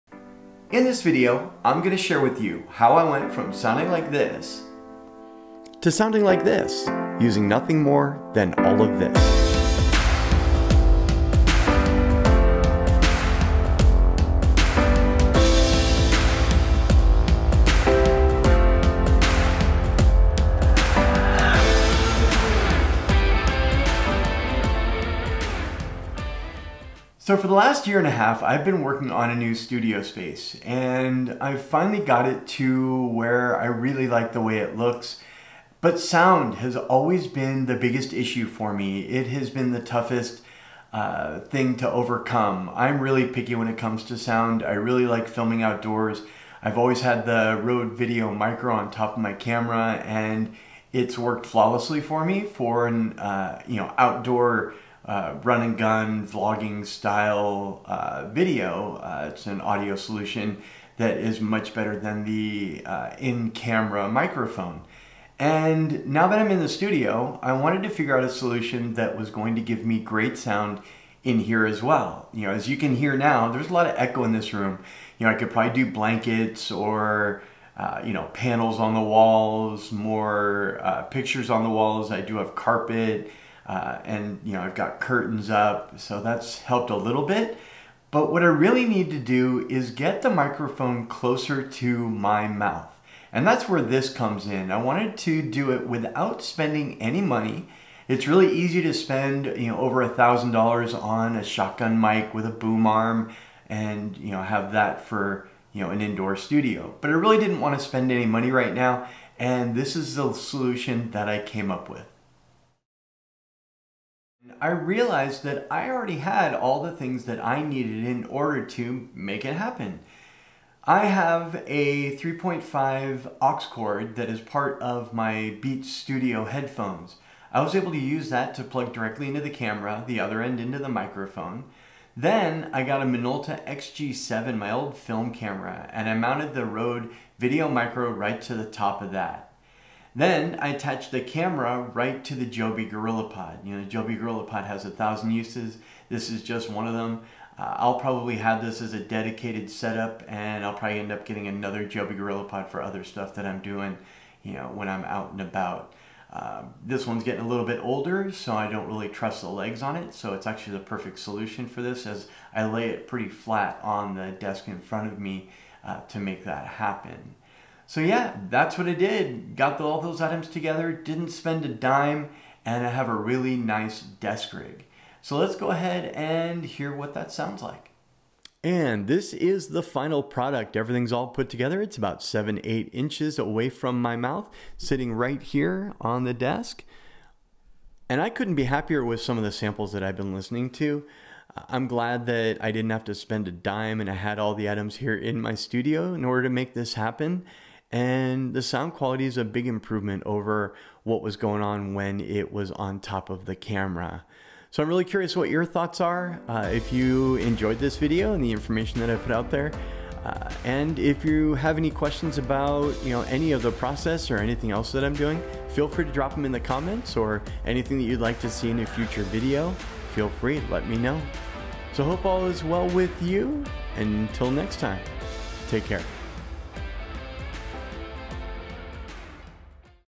Rode Video Micro, YouTube Studio Setup!
Today I talk about upgrading my audio without spending any $!